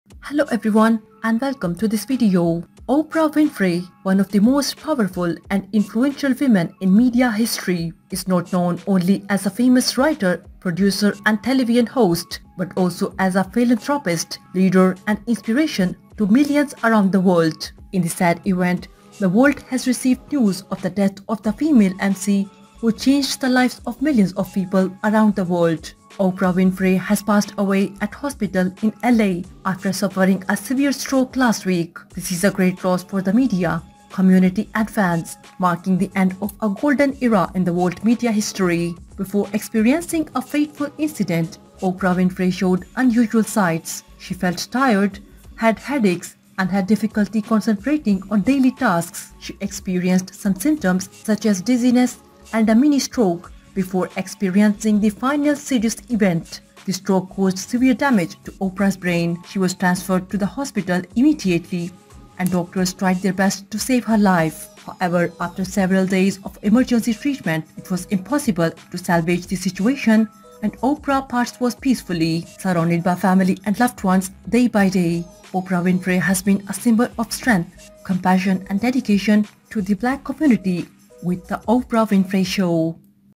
Today R.I.P! Oprah Winfrey's Tears Of Grief // She Passed Away Today And My Life Will Never
Oprah Gail Winfrey, also known mononymously as Oprah, is an American talk show host, television producer, actress, author, and media proprietor. She is best known for her talk show, The Oprah Winfrey Show, broadcast from Chicago, which ran in national syndication for 25 years, from 1986 to 2011.